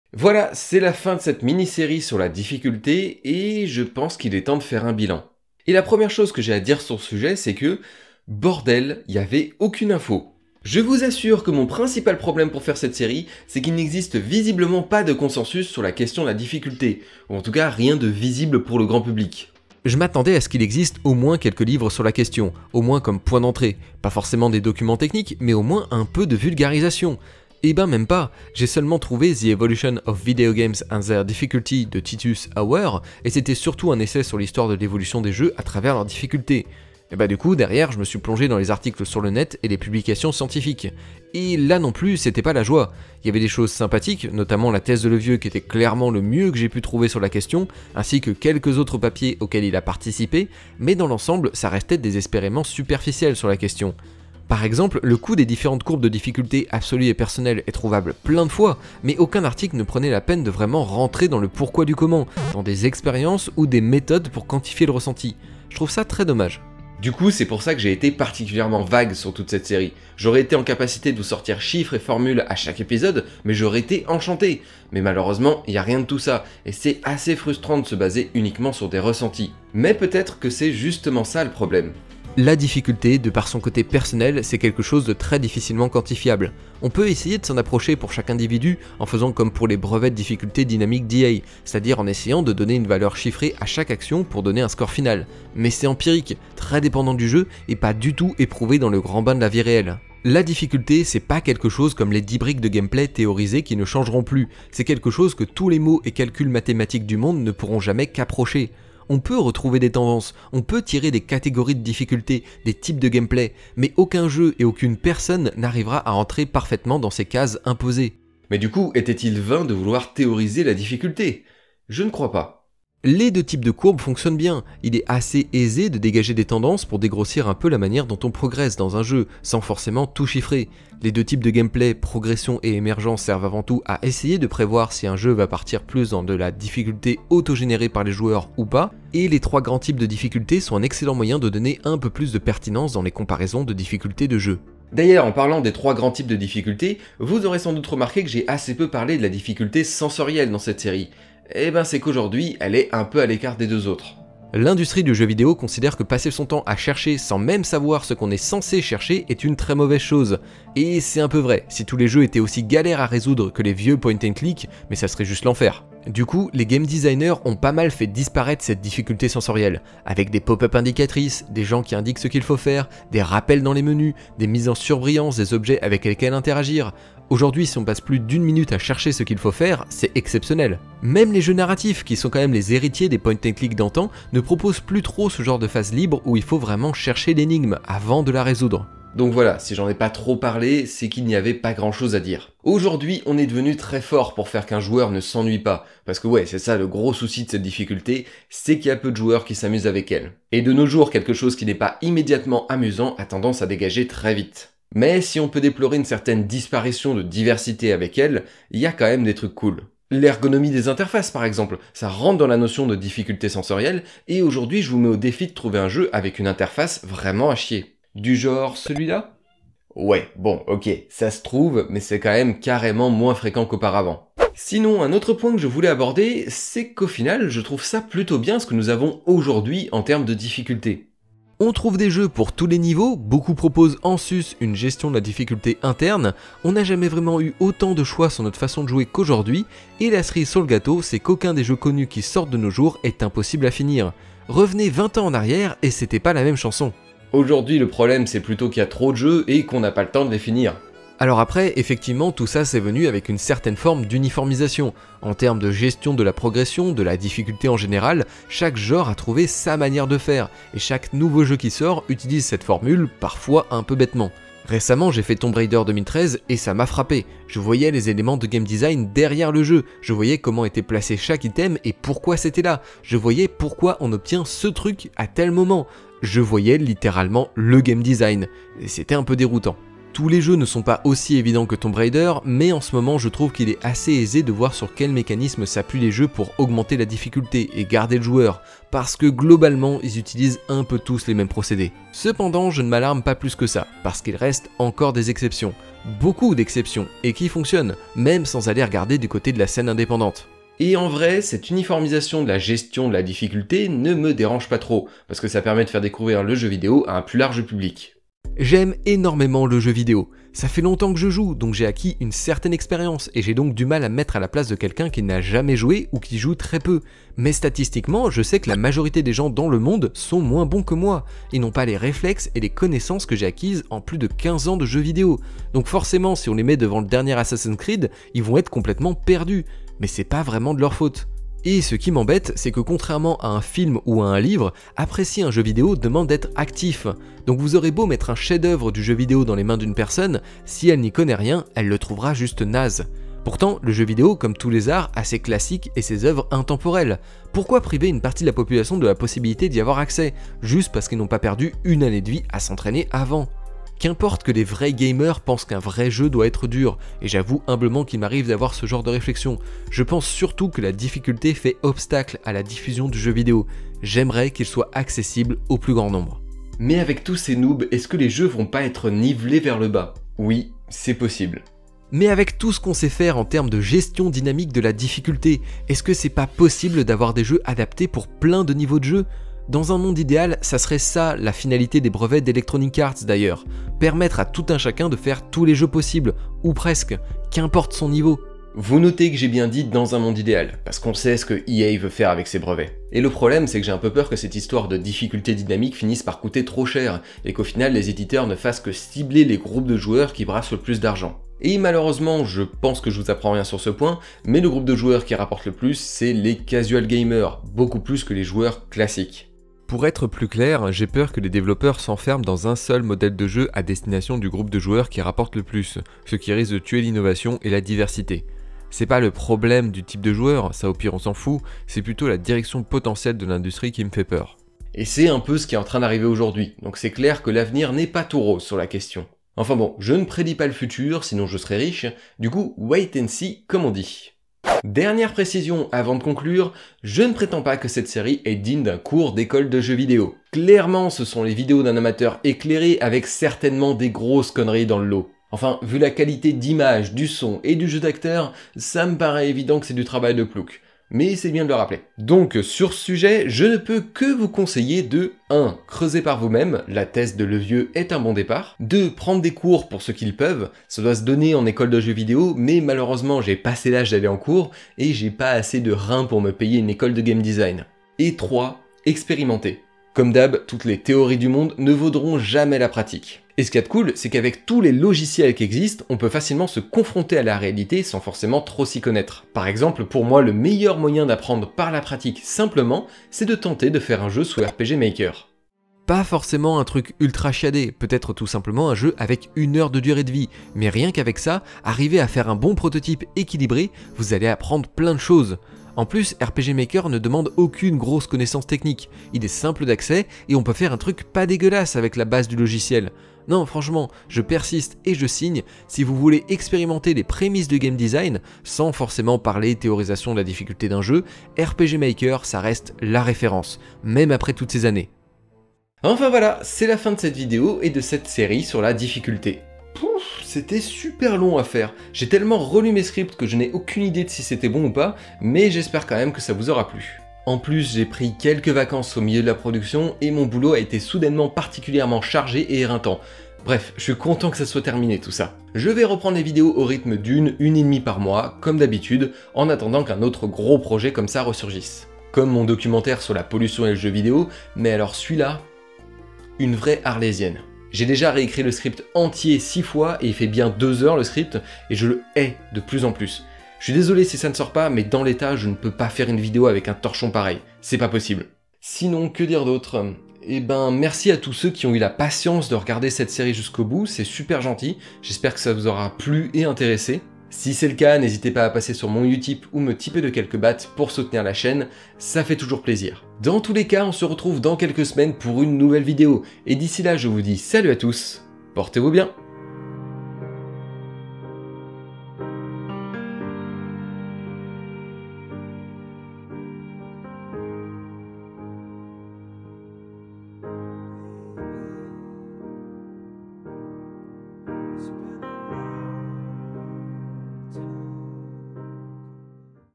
La difficulté dans le jeu vidéo #5 - Vrac & Conclusion
Cinquième et dernier épisode de ma série sur la difficulté dans le jeu vidéo ! Pour cette dernière partie je vous propose de parler de deux trois points que je ne savais pas trop ou caler, et de faire une petite rétrospective sur la série. J'espère que ça vous plaira !

La thèse de Levieux qui m'a servi de grosse base pour cette série

# Infos utiles #